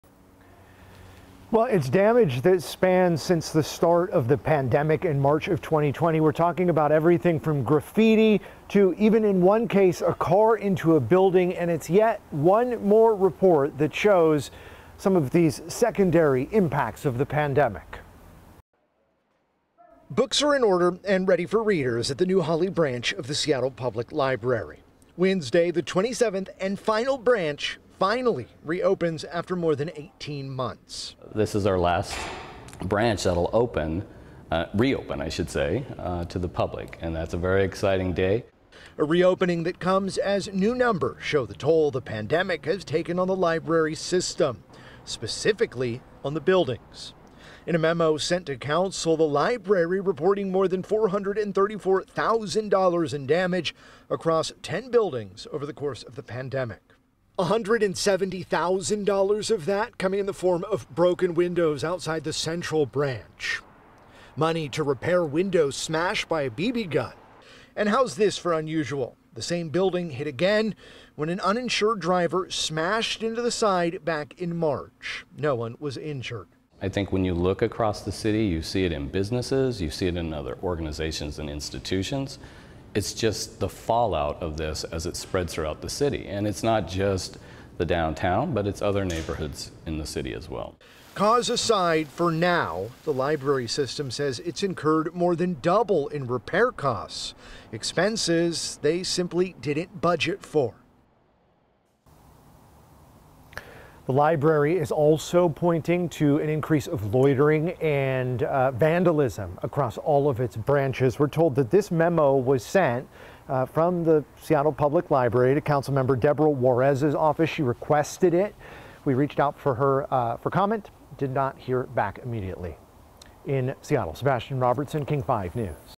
Seattle Public Library branches rack up more than $434,000 in damage during pandemic
Seattle Public Library (SPL) administrators say the damage incurred over the course of the COVID-19 pandemic to its facilities more than doubles that of a normal, non-pandemic year, totaling more than $434,000.

SPL provided a breakdown of the costs in a budget report titled, "Cost Incurred or Estimated due to Vandalism," which shows the staggering toll the pandemic has taken.

According to the report, $170,000 of the total $434,188 in damages came from broken windows at the Central Library Building in downtown Seattle.